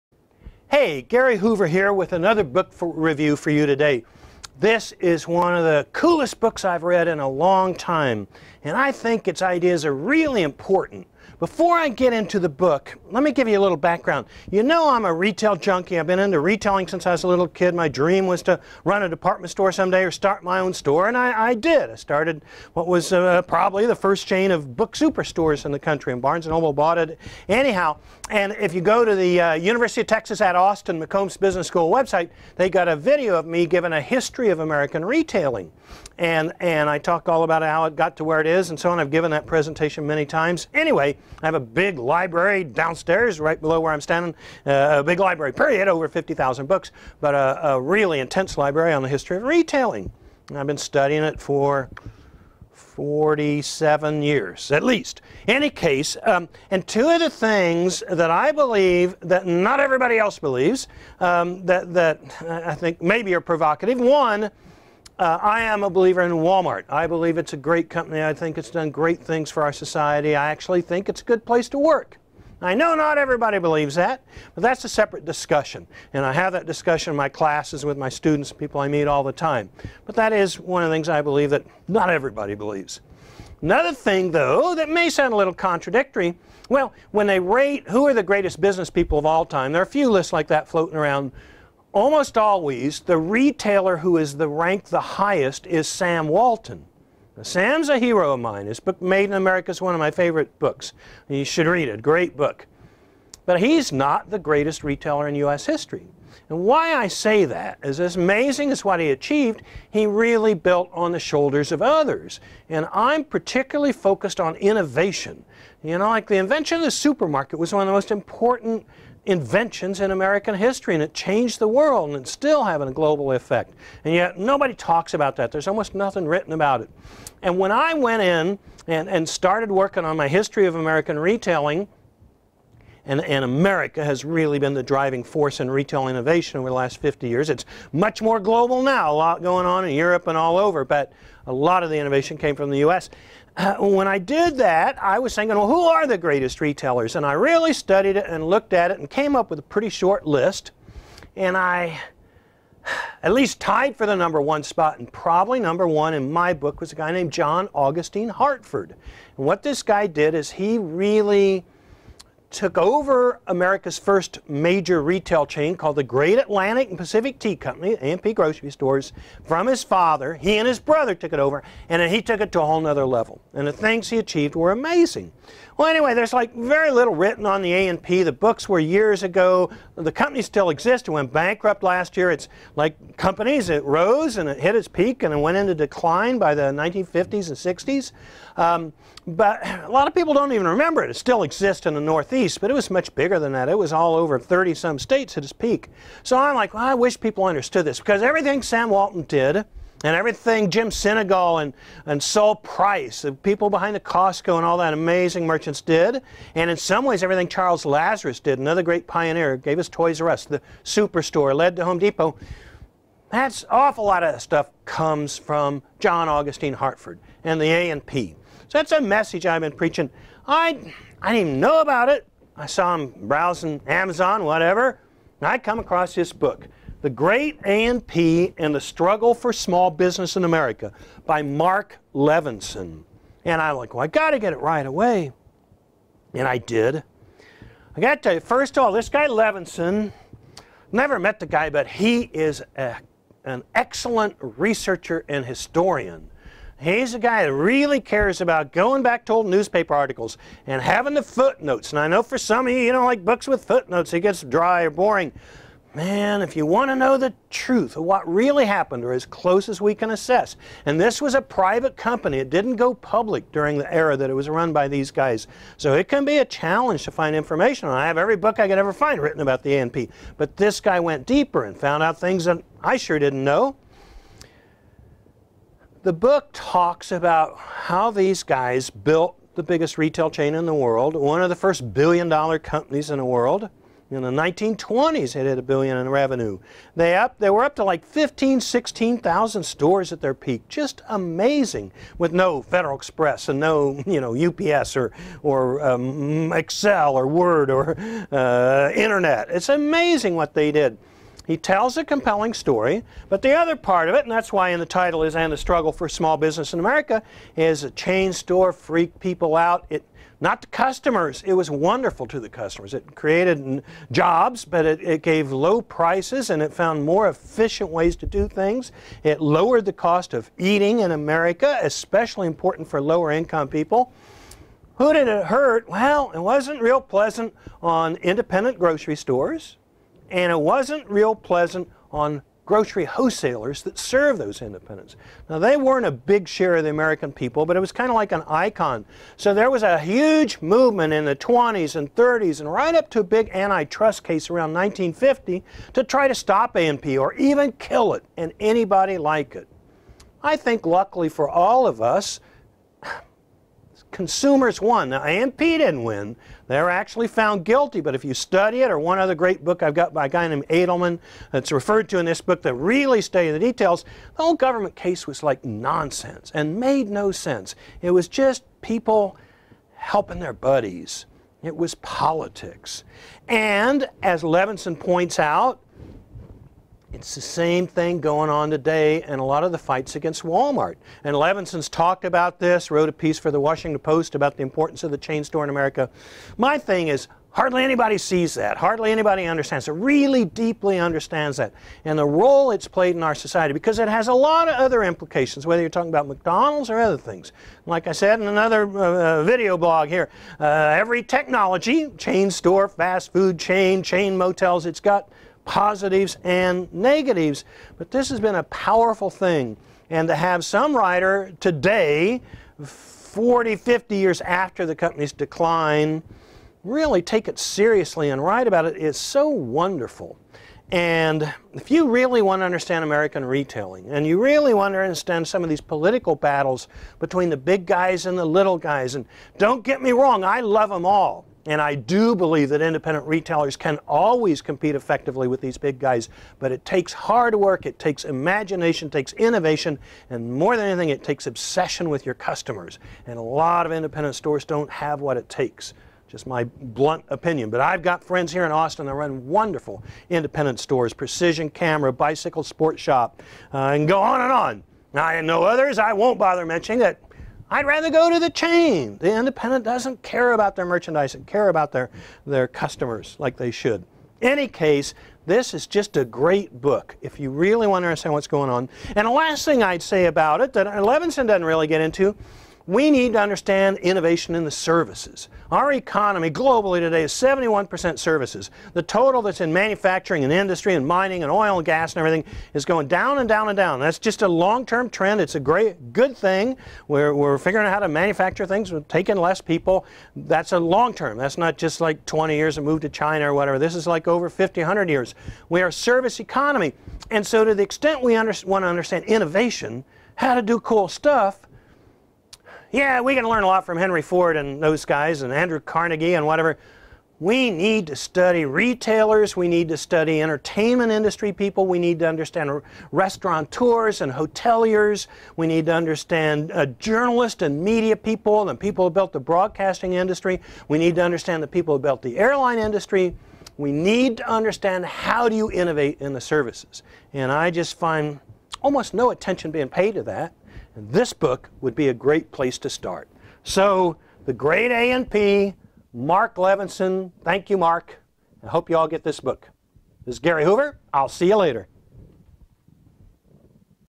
Gary Hoover Video Book Review Most Important Retailer A&P
I think we need to study innovation in the service industries, which will dominate our global economy more and more in the coming decades.  Very little research is done in this area.  I am a lifelong student of retailing, one of the biggest service industries.  For anyone who wants to understand retailing, big business, innovation, politics used to try to stop progress, this new book is required reading. It's one of the most important books in a long time, finally telling a story that has not received its due.  Kudos to author Levinson.